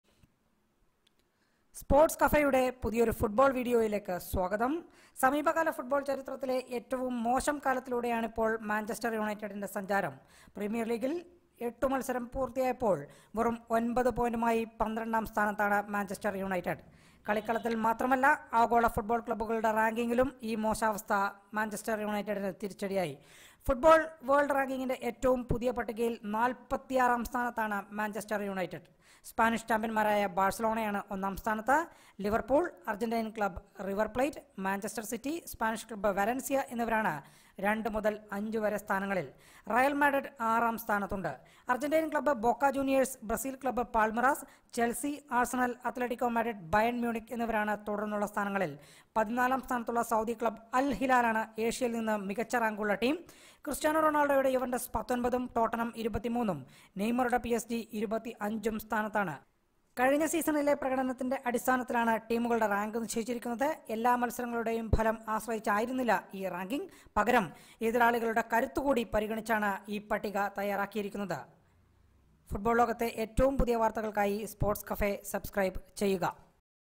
ലോക റാങ്കിംഗ്: ബാഴ്സലോണ ഒന്നാംസ്ഥാനത്ത്; യുണൈറ്റഡ് നാൽപ്പത്തിയാറിലേക്ക് വീണു
Manchester United are ranked the 46th best team in the world out of top 50 clubs, according to Football World Rankings. The Red Devils'worst performance in the Premier League in 30 years is the cause for their drop in the ranking. Saudi Arabian side Al Hilal, Chinese Super League club Guangzhou Evergrande and a host of teams in Europe and other continents are ranked above the Old Trafford outfit.

Barcelona, Liverpool, River Plate, Man City and Valencia occupy the top 5 spots while 13-time European champions, Real Madrid dropped to sixth. Chelsea and Arsenal occupy tenth and 11th position respectively while Juventus are good for 19th, Tottenham in 23rd and PSG are ranked 25th.